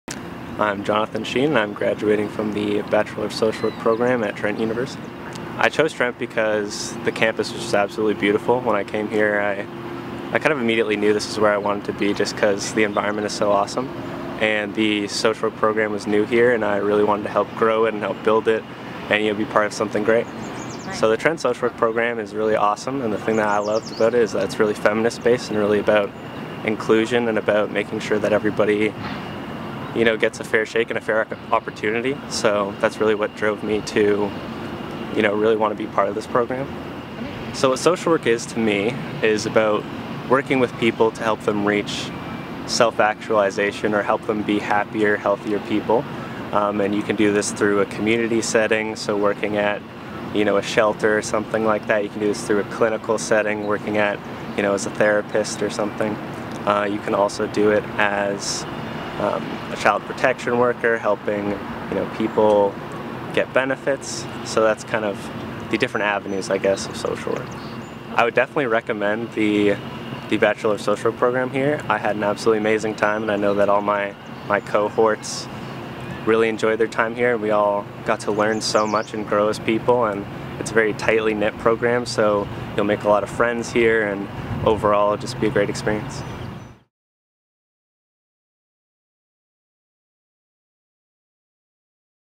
Convocation 2018 Spotlight - Johnathan Sheehan, Bachelor of Social Work Honours
TrentUGrad Johnathan Sheehan talks about his experience in Trent University's Bachelor of Social Work Honours program.